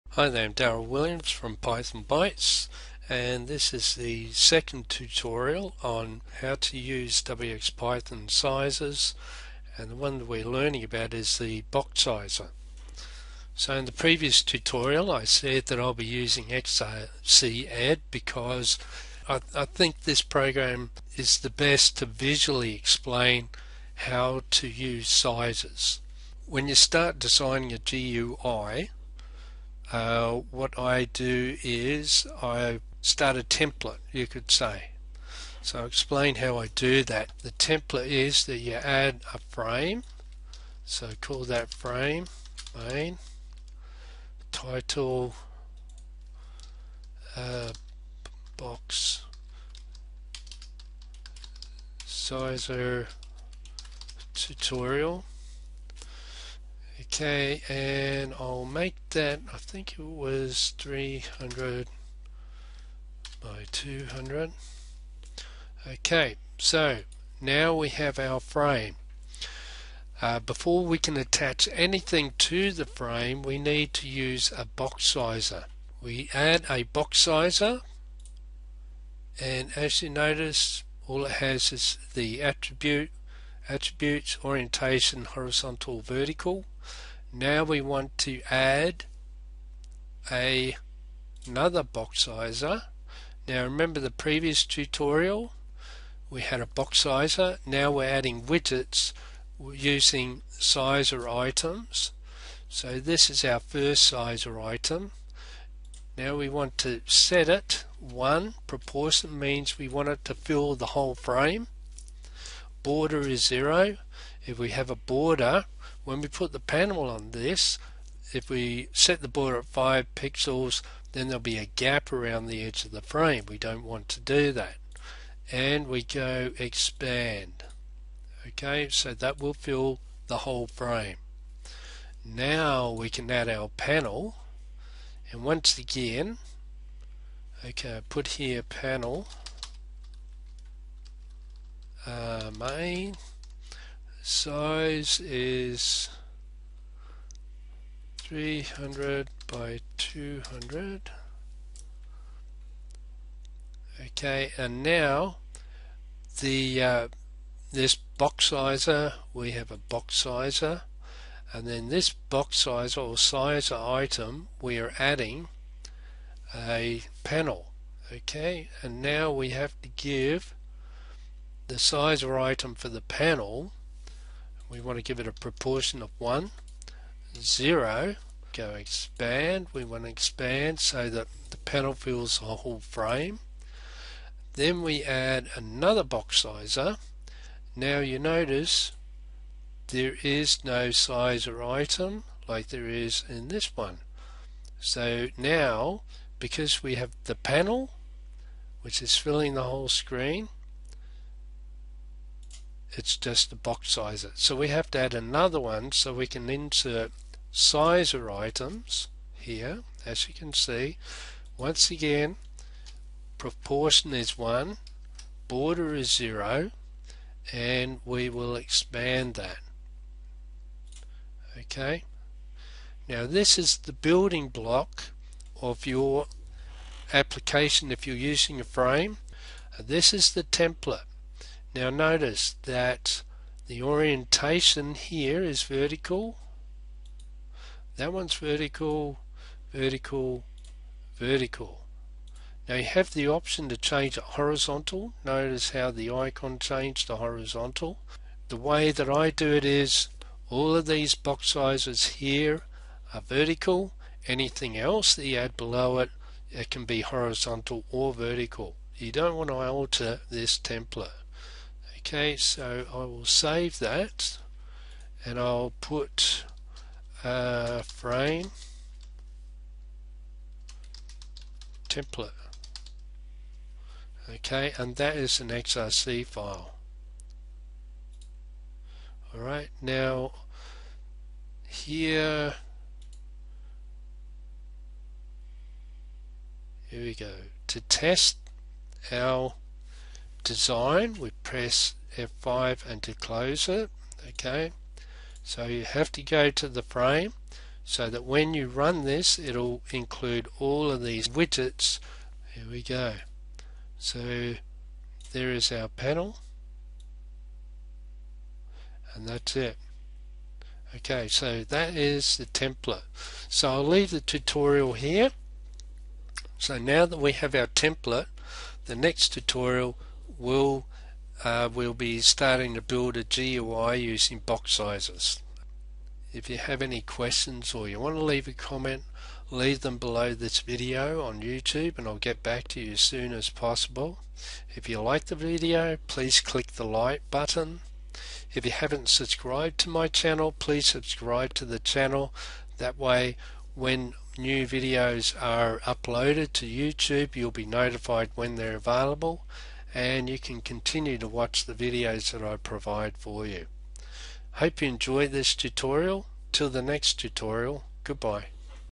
wxPython Boxsizer Explained 2
In this tutorial I explain how to create a frame template with boxsizers. You will learn why you want a template for future applications. This means all your wxPython GUI applications will start the same. Simplifying your design and building procedures.